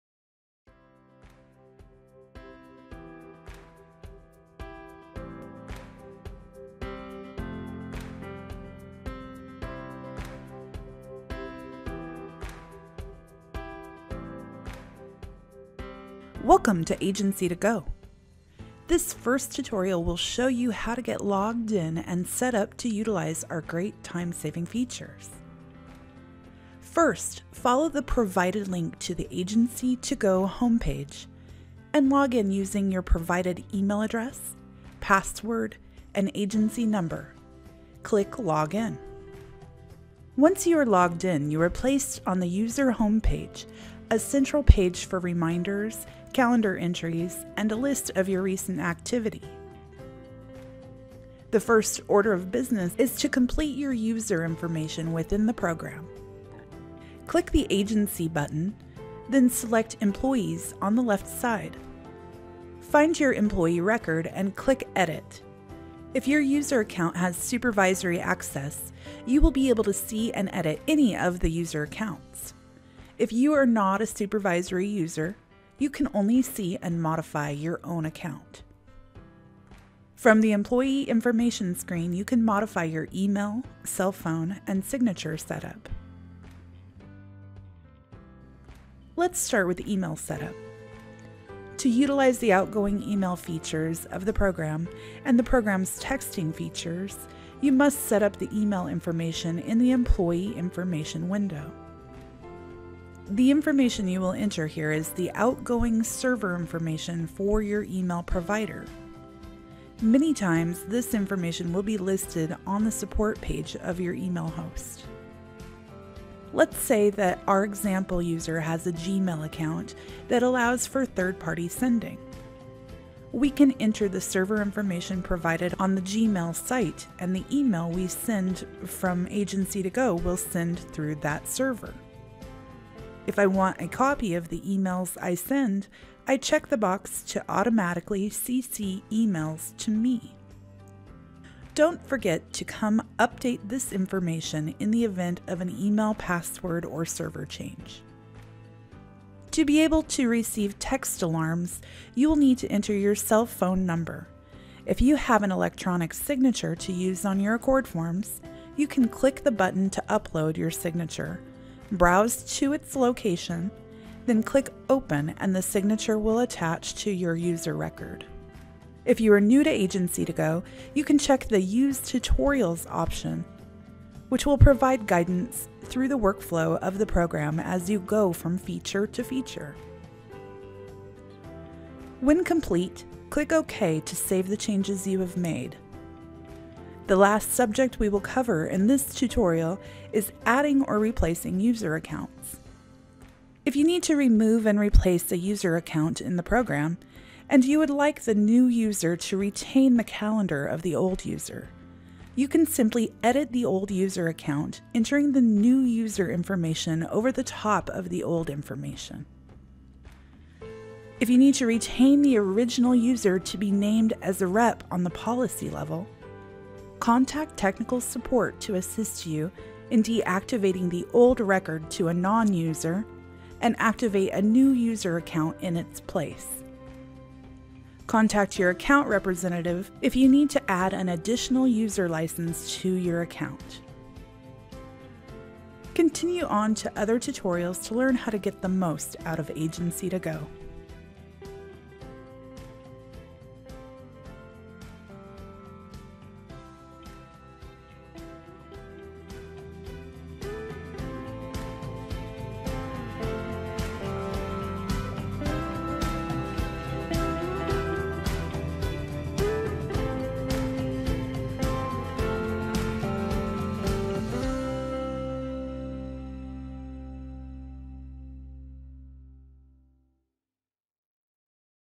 Setting Up your User Account
This video shows how to log into Agency2Go and configuration of your employee information.